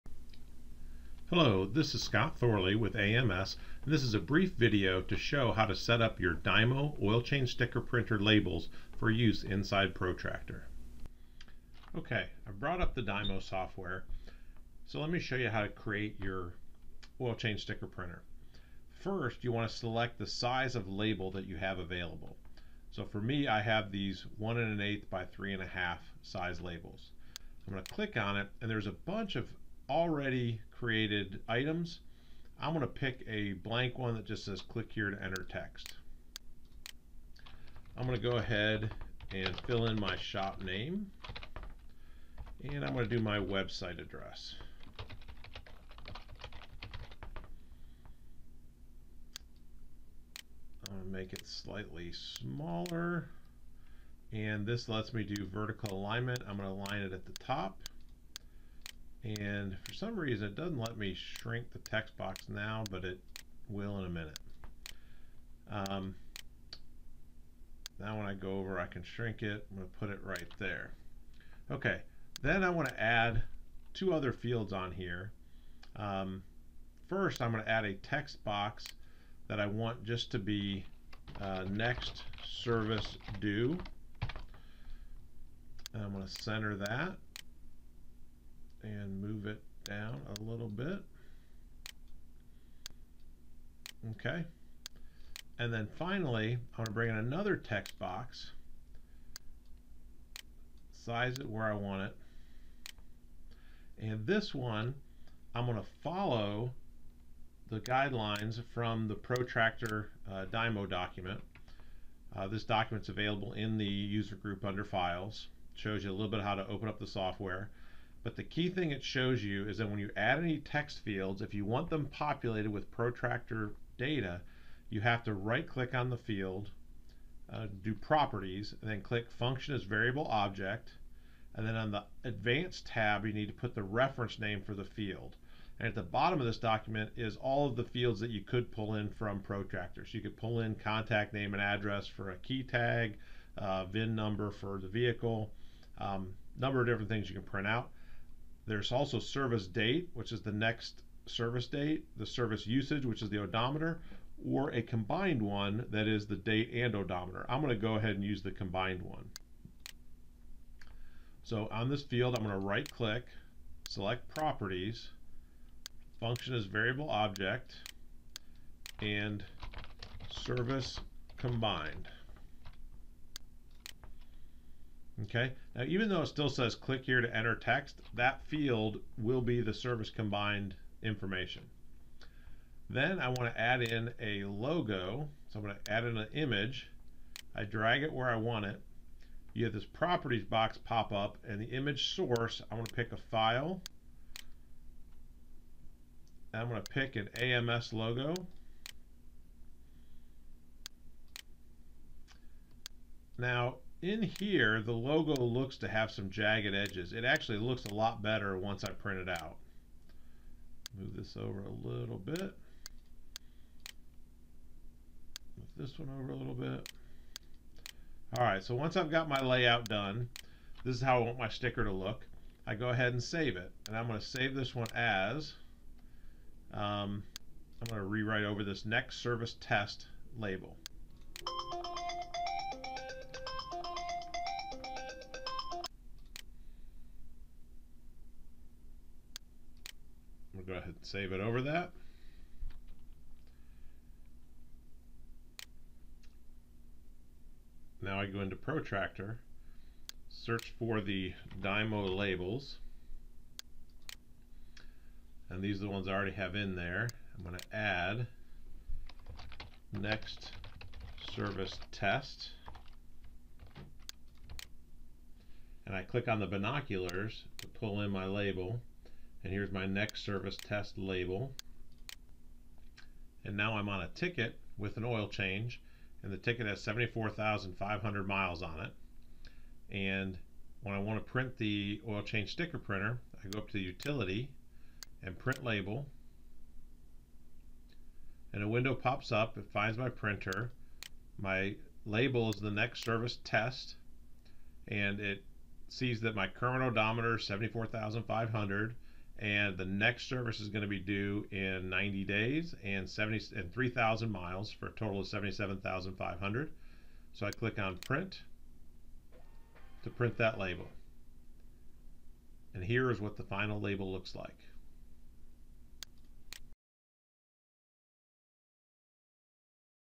Dymo OCSP
How to create Dymo Oil Change Sticker labels for use in Protractor